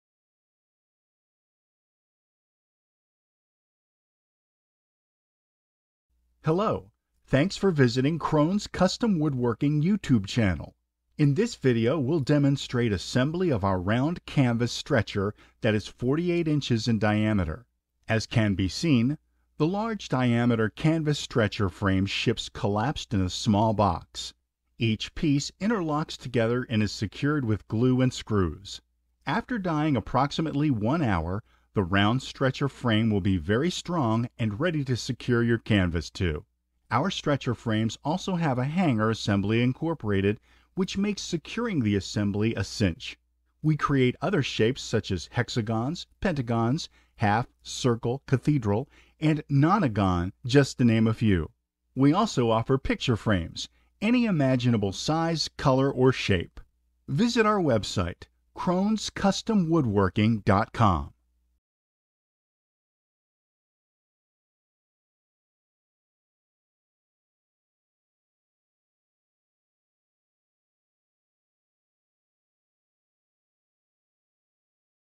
Assembly of round canvas stretcher frame by Crone's Custom Woodworking
Our round canvas stretcher - canvas strainer ships disassembled and goes together like puzzle pieces. The collapsed design made of birch plywood makes our product really strong and economically feasible.